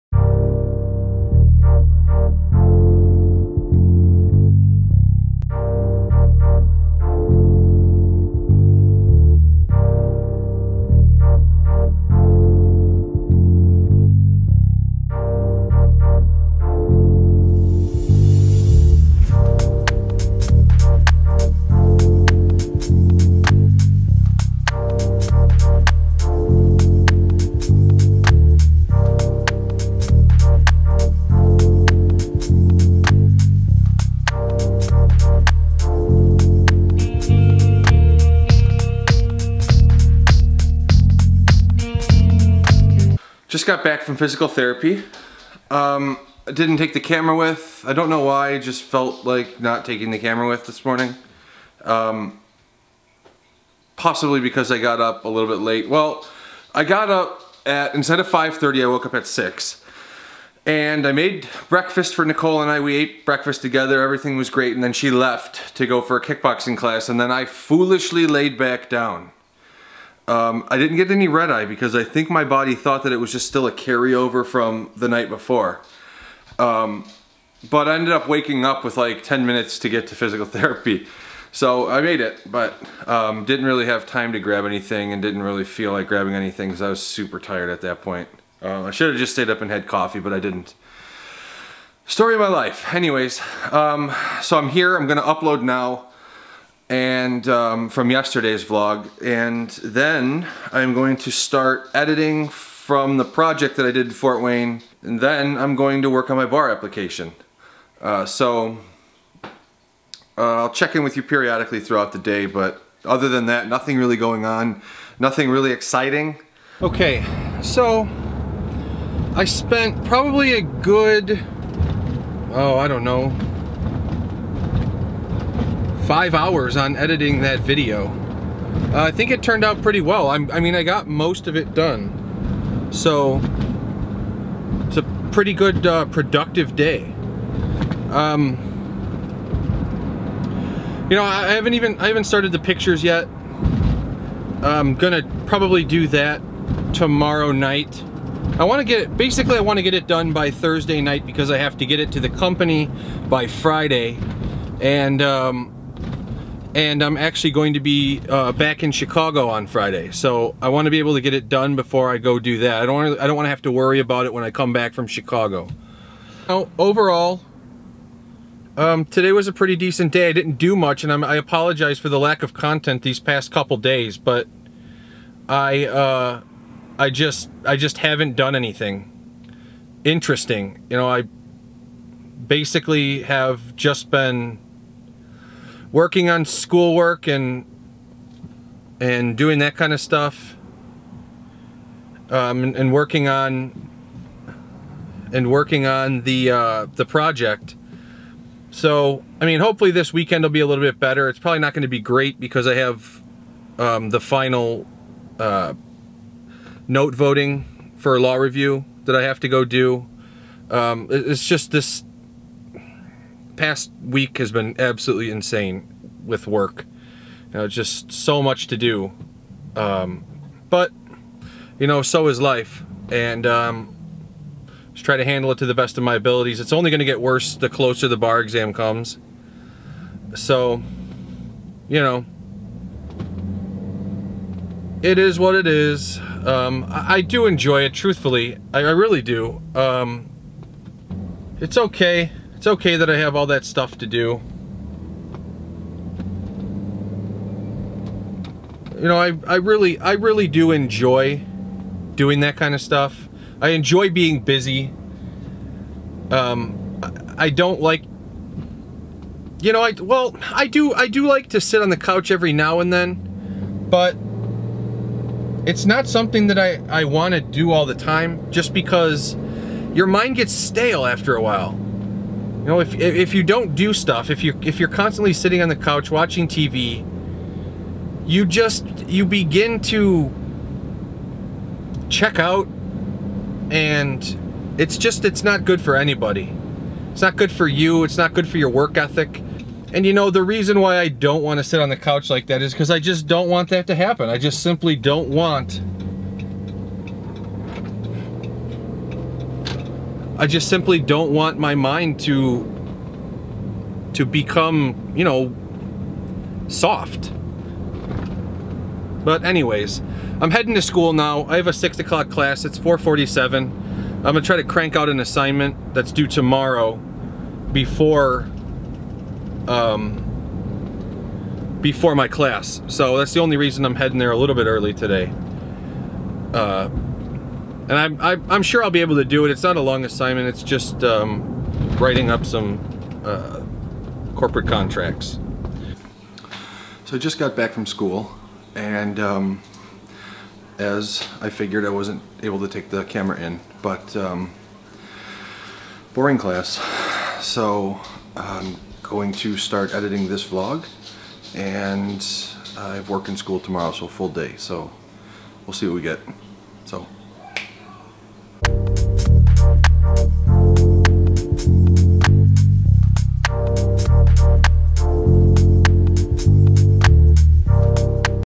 Video Projects and Couch Surfing!
I am not a proponent of couch surfing.  I think that the human body and mind cannot improve by just sitting on the couch watching tv.  I love to be busy and to have my mind on multiple different things at the same time.  I think it is important to constantly be busy and always be thinking.  The mind thrives on this.  Anyways, have a great day.  Stay Positive!!!